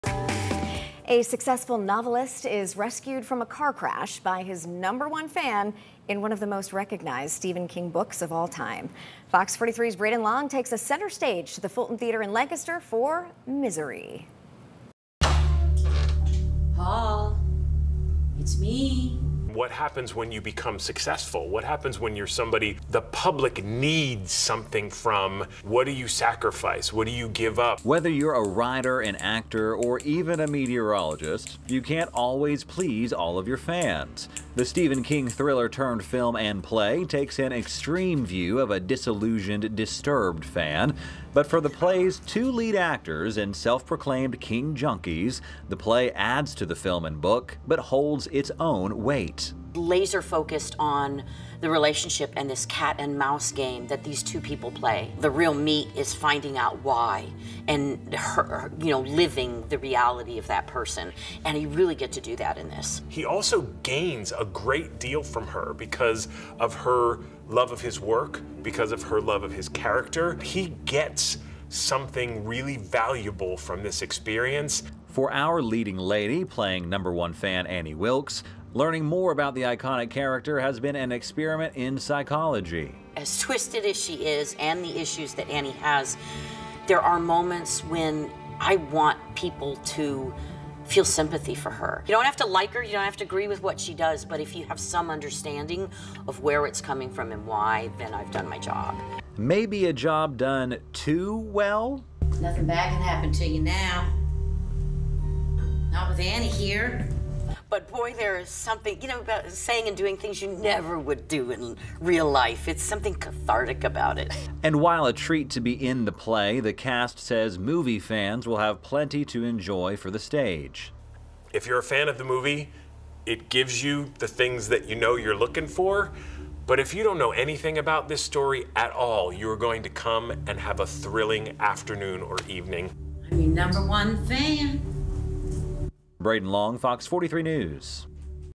The power of obsession in Stephen King classic 'Misery' | Center Stage
Whether you're a writer, perhaps an actor, or anyone in the public eye, you can't always please all of your fans.

The Stephen King thriller-turned-film and play "Misery" takes an extreme view of a disillusioned, disturbed fan in the horror classic. But for the two lead actors in the show, the play holds its own weight compared to the movie and book.

"It's laser-focused on the relationship and this cat-and-mouse game these two people play," said Charis Leos, who portrays the iconic villain Annie Wilkes in the show.